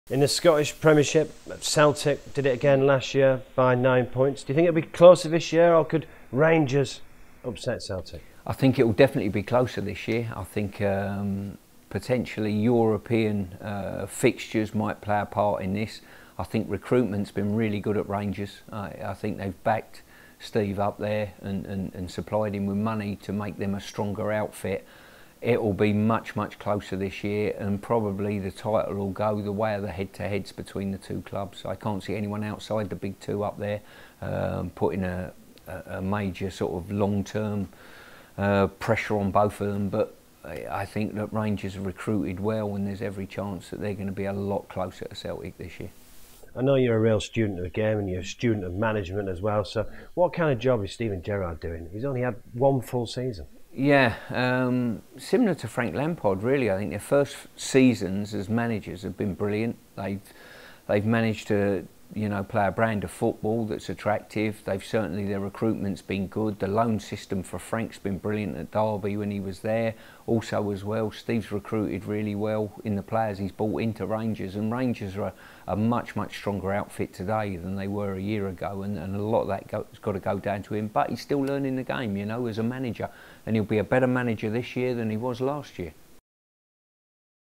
2019/20 Season preview | Stuart Pearce on Rangers and Celtic
Ahead of the start of the new football season, former England Captain Stuart Pearce joined BetfredTV to look at north of the border at Rangers and Celtic